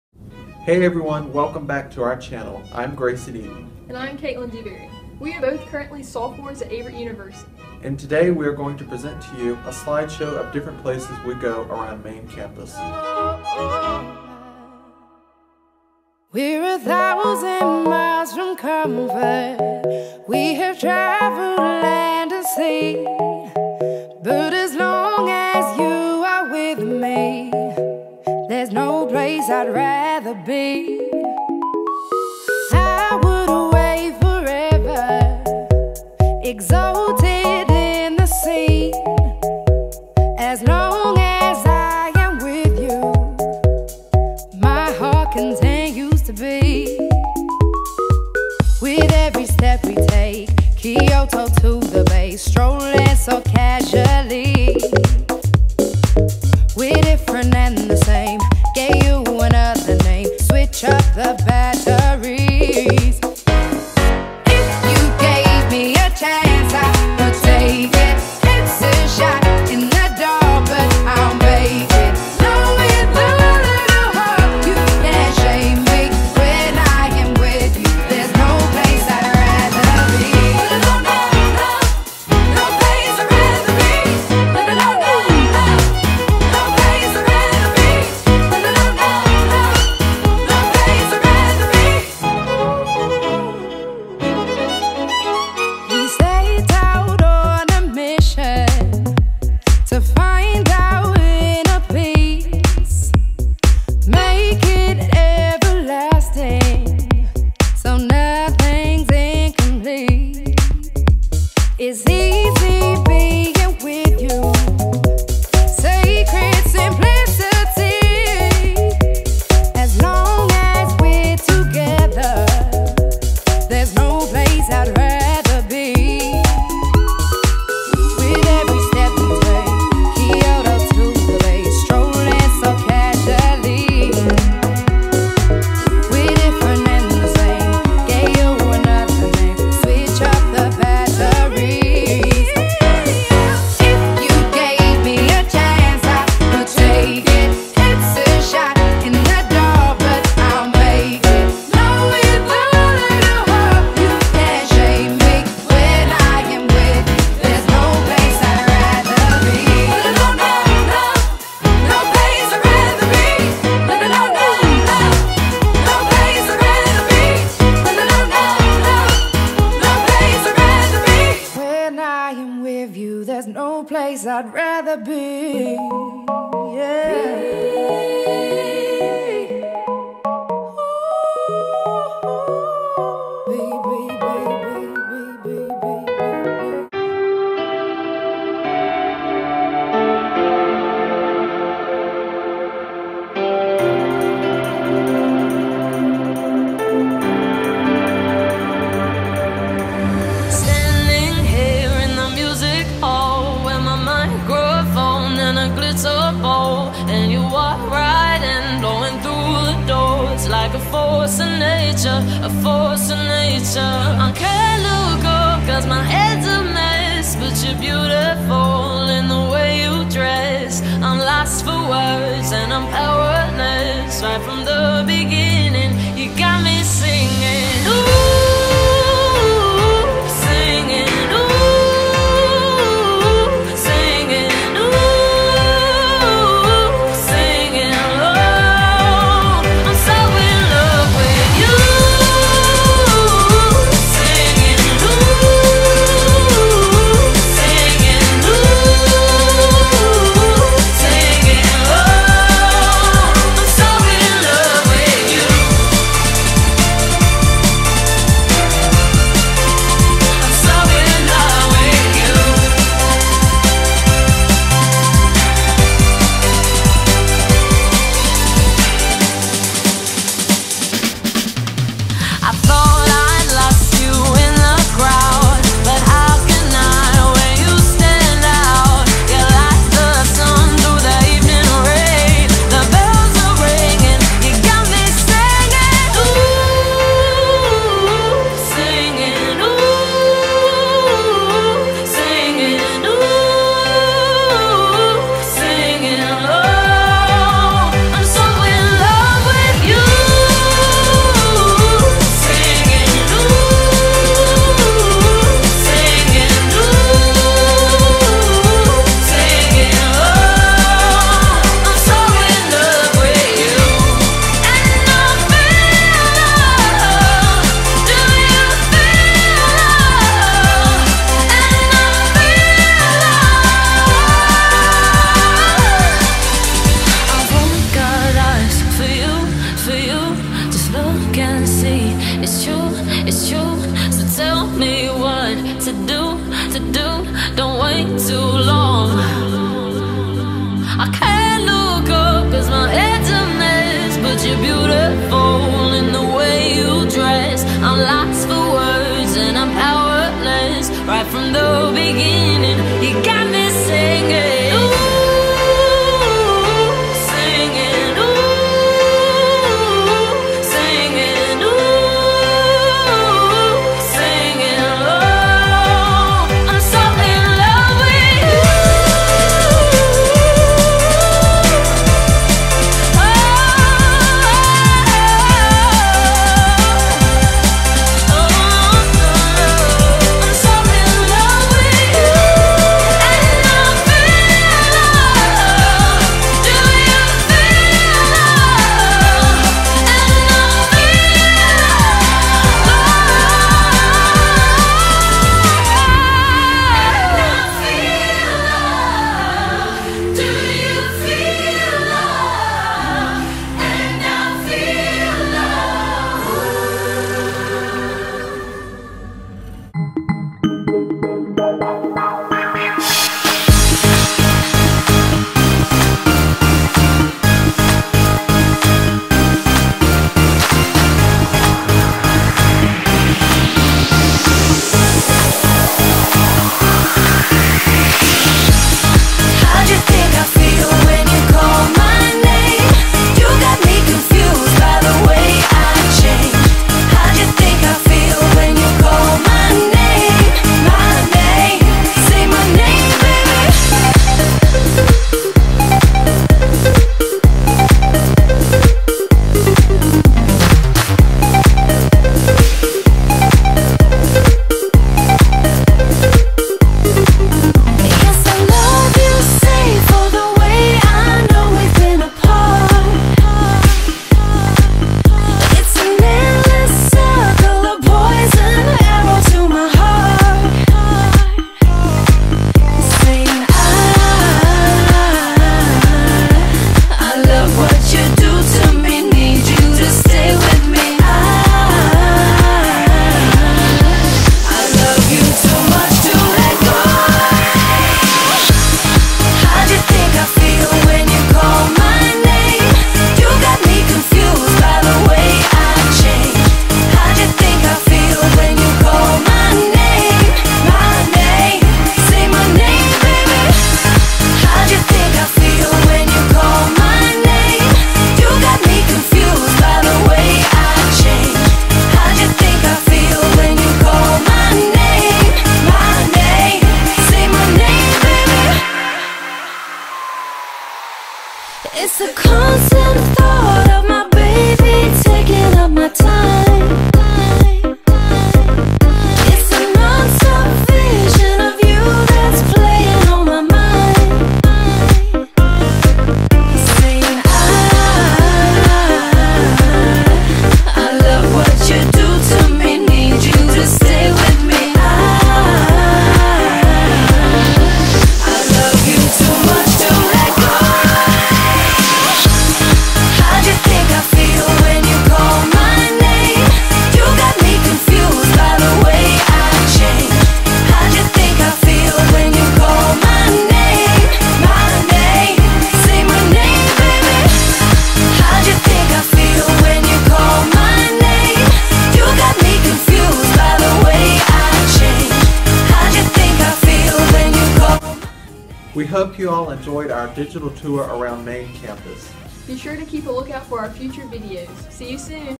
Digital Tour of Averett University
A short video of various places on main campus.